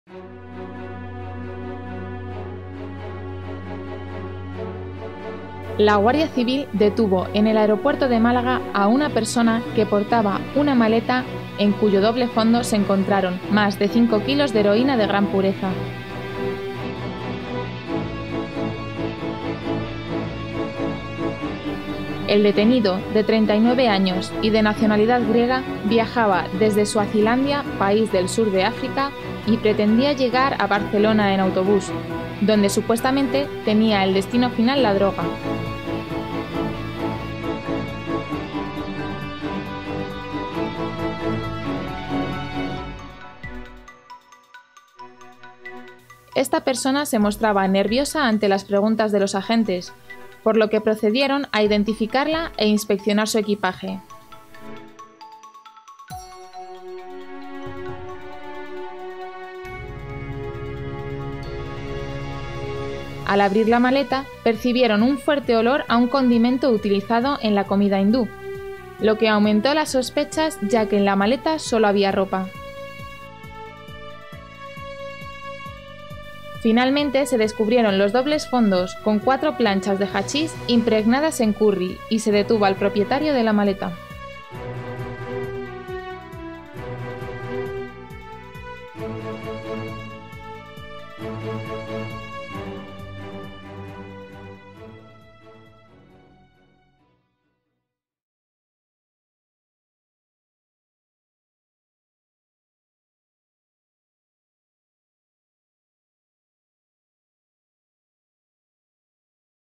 Detenida en el aeropuerto de Málaga una persona que intentaba introducir heroína.
Agentes pertenecientes a la Sección Fiscal (ODAIFI) de la Guardia Civil del Aeropuerto de Málaga han detenido a una persona que portaba, en el doble fondo de su maleta de viaje, más de 5 kilos de heroína de gran pureza.
Se trata de un individuo de 39 años y nacionalidad griega que viajaba a Málaga vía Doha, procedente de Suazilandia, un país situado al sur del continente africano. Pretendía continuar su viaje en autobús hasta Barcelona, donde supuestamente tenía el destino final la droga.
Esta persona infundió sospechas a los agentes a su llegada al aeropuerto, al mostrarse nervioso ante las preguntas que éstos le formularon sobre los motivos de su viaje, por lo que los guardias civiles le identificaron e inspeccionaron su equipaje.
Al abrir la maleta que portaba, los agentes percibieron un fuerte olor a un característico condimento utilizado en la comida hindú, lo que aumentó sus sospechas ya que en el interior de la misma tan solo había ropa, por lo que decidieron revisar la maleta con detalle ayudados de un scanner.
Durante la inspección de la maleta descubrieron dobles fondos en los que hallaron cuatro planchas impregnadas en curry,  con una clara intención de confundir a los perros de los grupos cinológicos policiales en caso de ser inspeccionada.
En el interior de las planchas había una sustancia granulada de color marrón que resultó ser heroína de gran pureza cuando los agentes aplicaron el correspondiente test de drogas y que arrojó un peso total de 5.519 gramos.